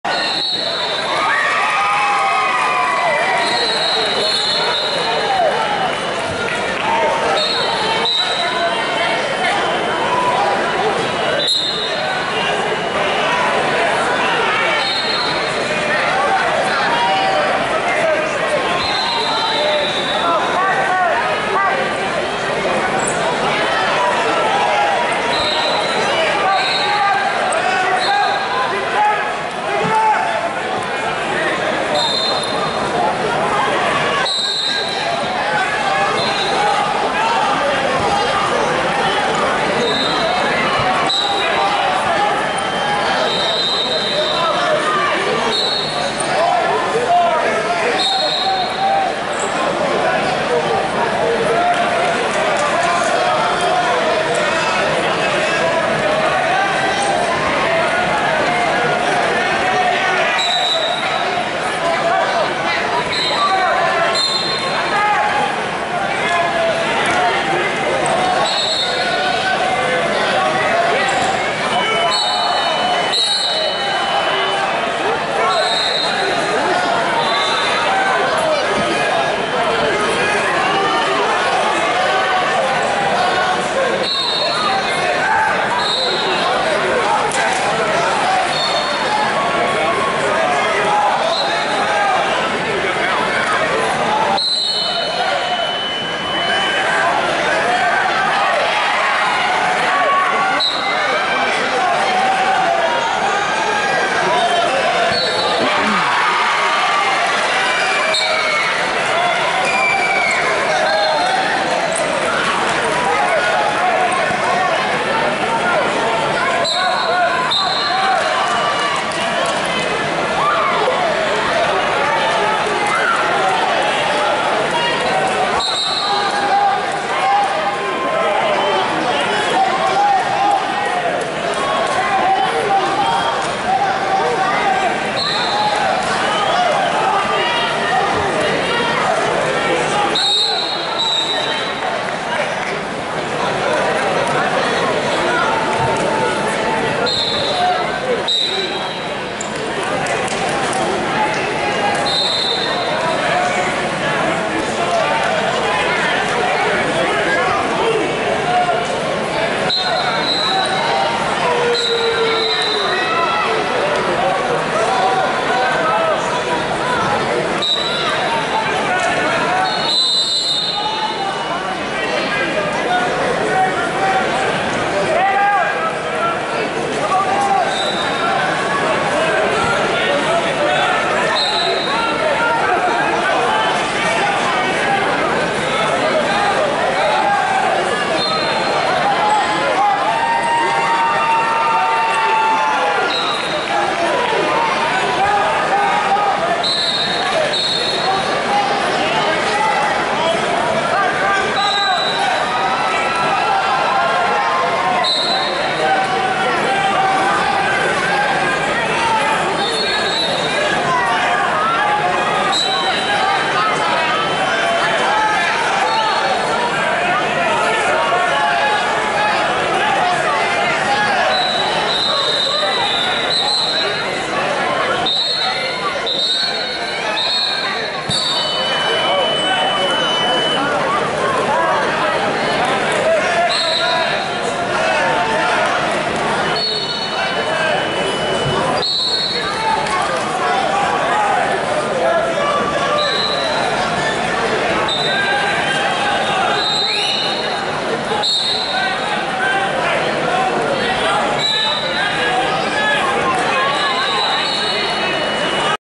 Devon Lane 2018 TSSAA State Championships vs Jefferson County
145 lb junior Devon Lane vs. Jefferson County in Consolation Round 1 at the TSSAA AAA State Championships at the Williamson County Agricultural Expo Center in Franklin, TN on Thursday, February 15th, 2018.